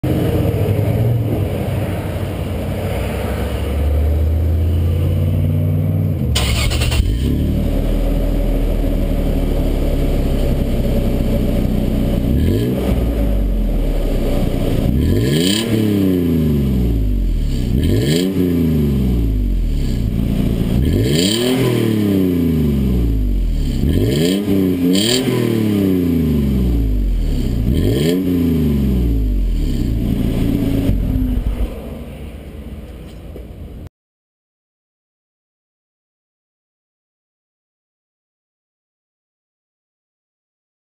MBRP Exhaust
Newly Installed MBRP Exhaust: It sounds much better with some weight and power behind it, but this will do.